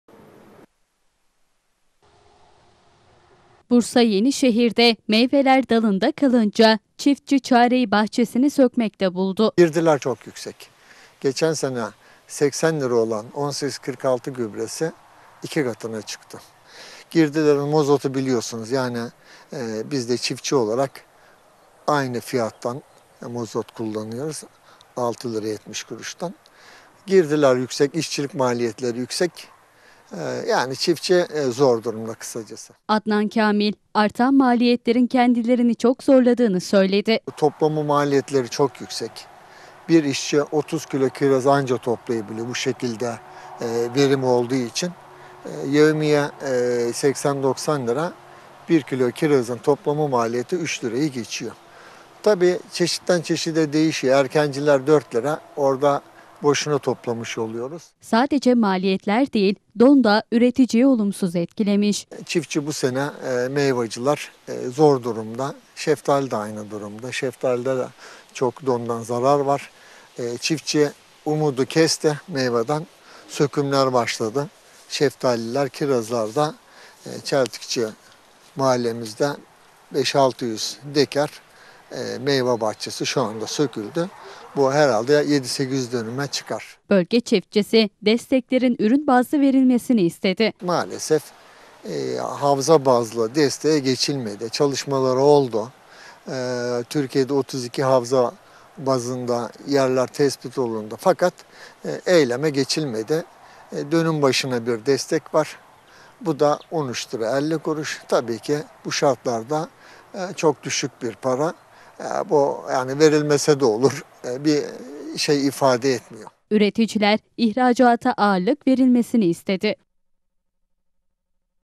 Çiftçi Çareyi Bahçesini Sökmekte Buldu
ÇİFTÇİDEN HABER

Türkiye'den son dakika haberler, köşe yazıları  bütün konuların tek adresi